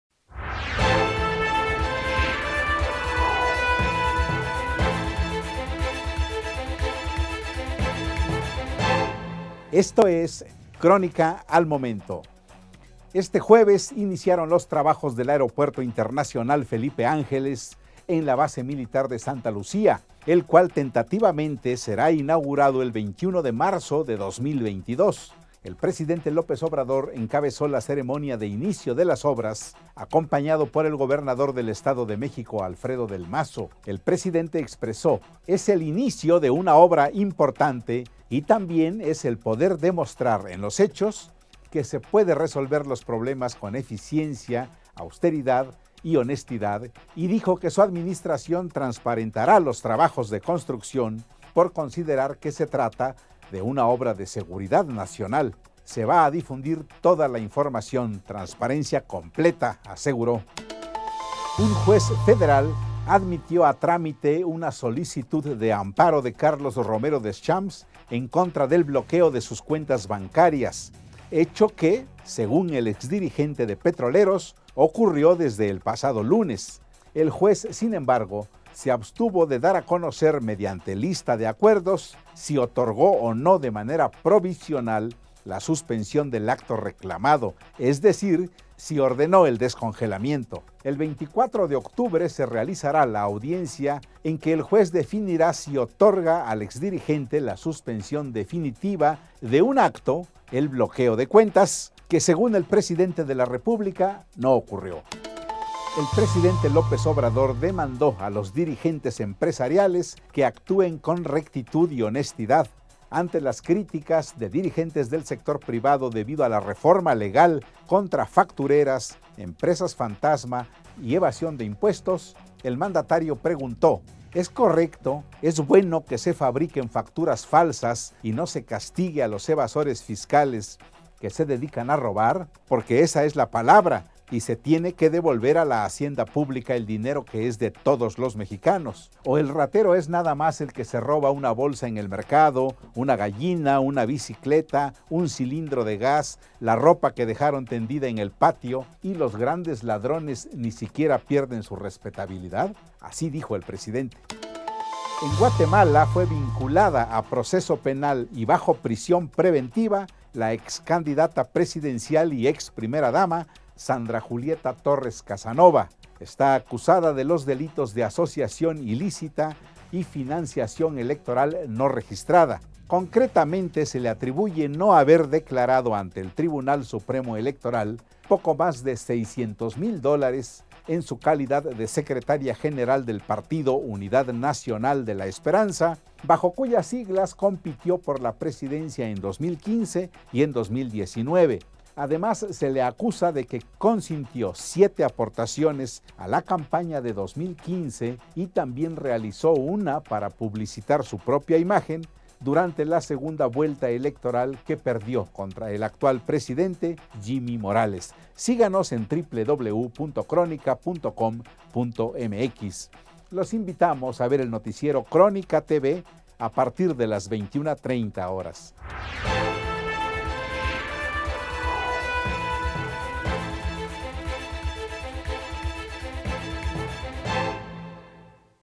Romero Deschamps ampara sus cuentas bancarias + Crónica Al Momento
► Romero Deschamps se amparó contra bloqueo de cuentas
► Dan prisión preventiva a excandidata de Guatemala